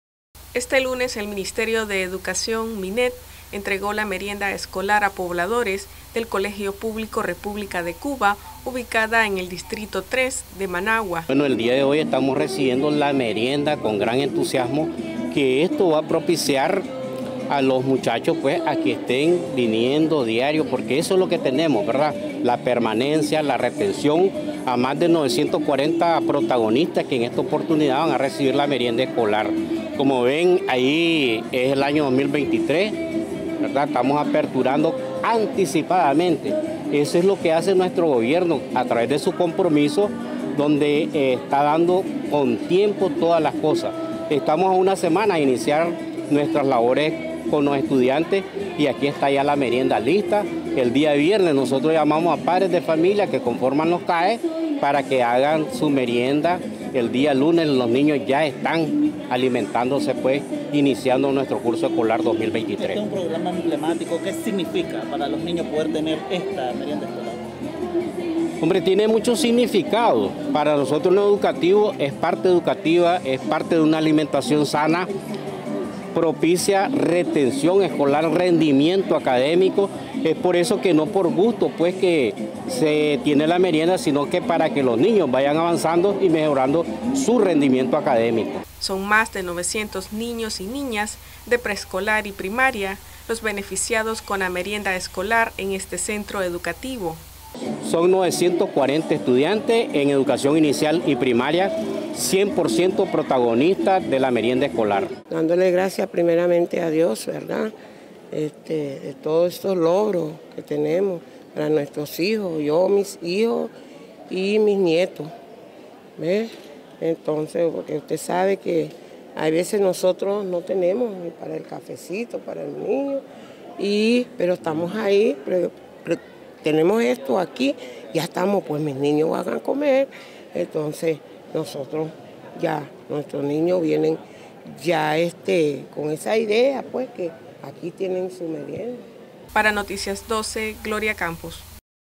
Noticias12Nicaragua Reciben Merienda Escolar en el Colegio Público República De Cuba del distrito 3
Pobladores del distrito III de Managua reciben la Merienda Escolar en el Colegio Público República De Cuba